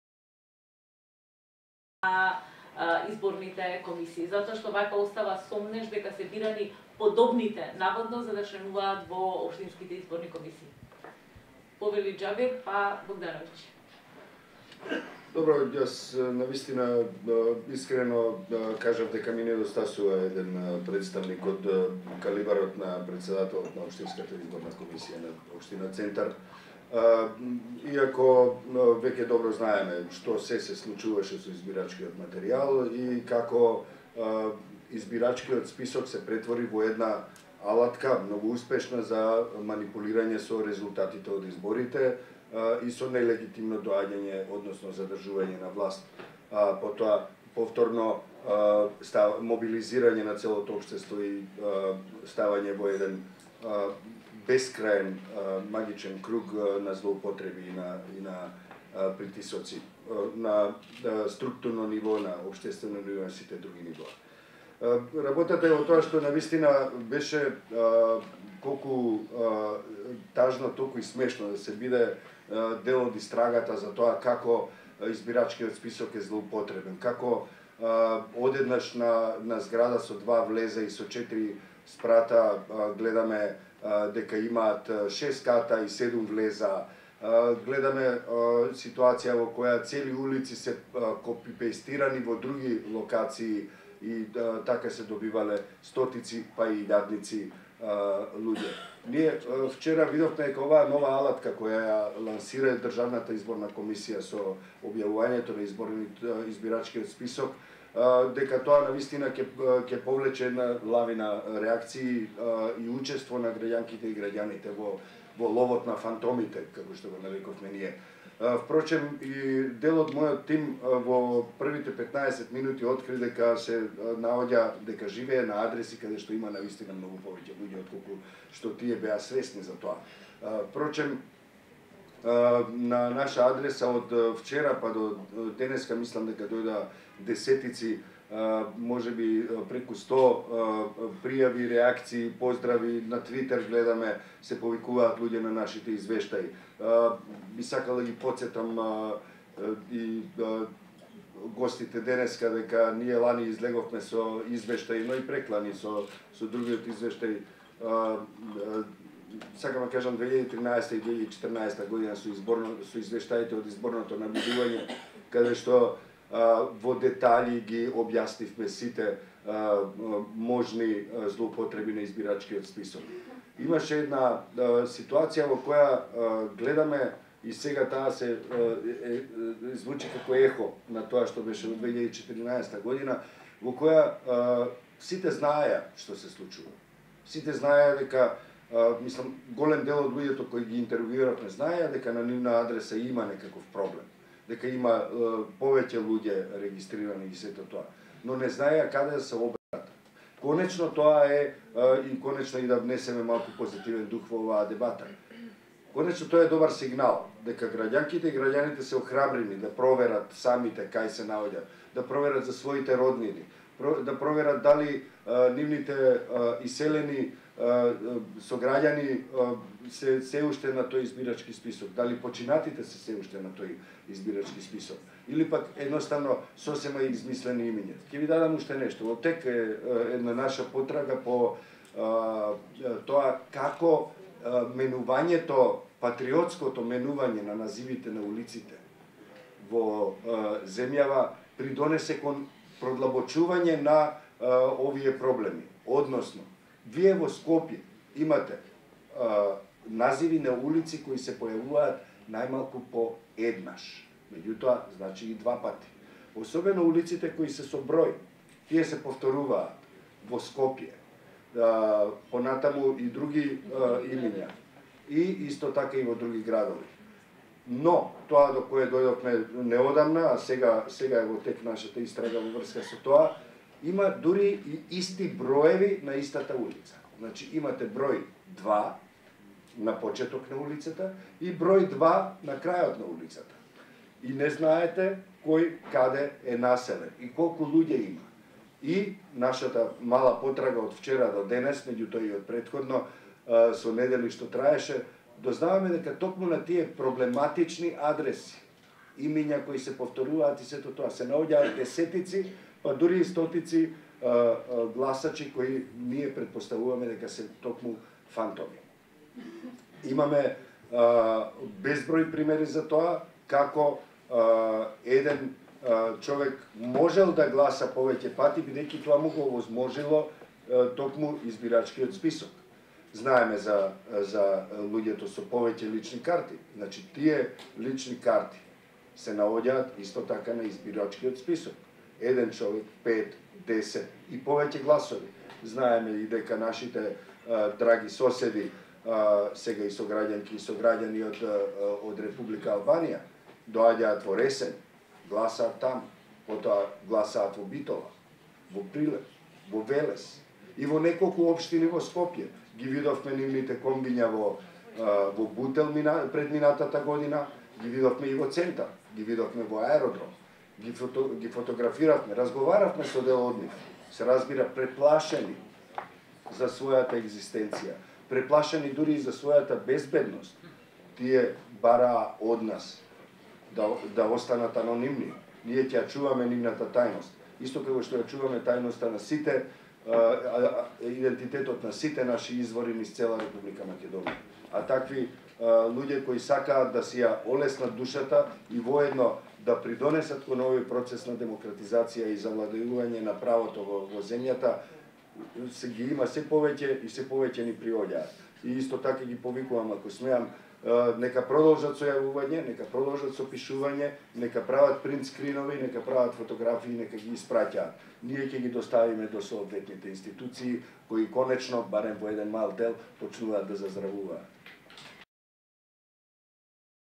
Дерала: Избирачкиот список беше алатка за изборни манипулации
Ви пренесуваме дел од излагањето на Џабир Дерала за тоа како Избирачкиот список бил злоупотребуван со цел да се спроведат изборни манипулации.
Дерала беше учесник на дебатата “Можни ли се фер и демократски избори на 5-ти Јуни, ќе се повтори ли 23 февруари на 6-ти Април?” која се одржа на 4 март 2016.